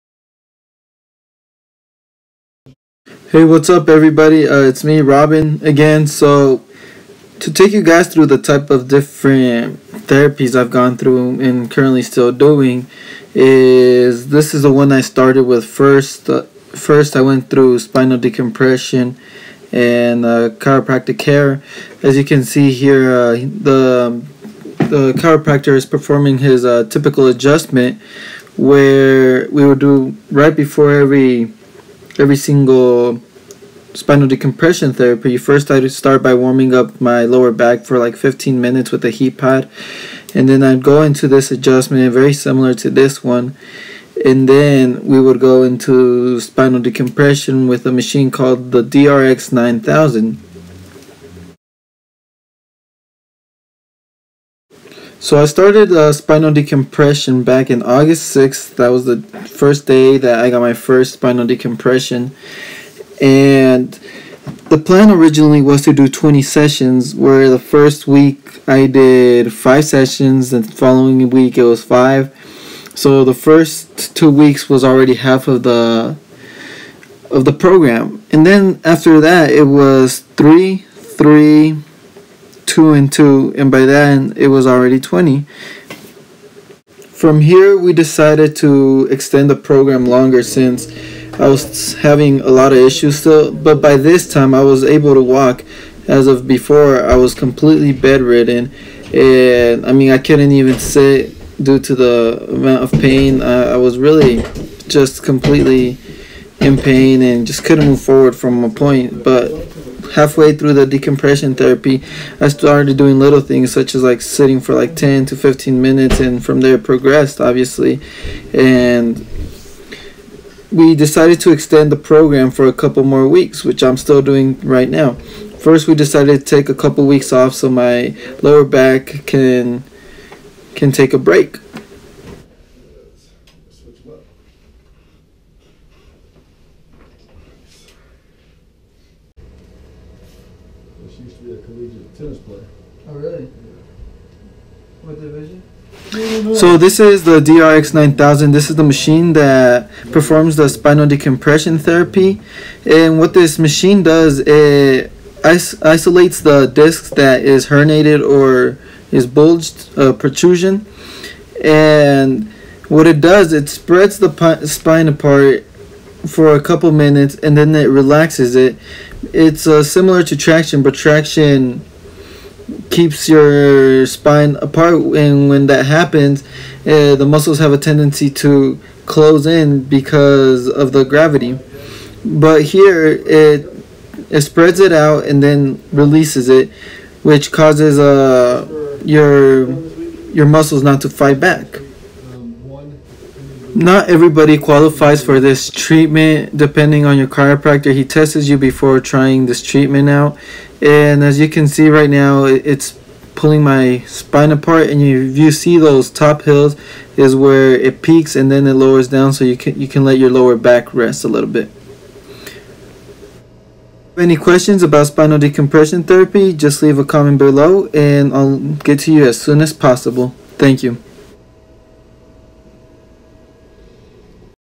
Herniated Disc/Sciatica Recovery: Part 2 (Spinal Decompression)
This is the first step I took during the recovery process. This is called spinal decompression. There is various machines that perform this function. These machines are meant to creates traction. With traction it causes the disc to create suction of the disc allowing it to go back in.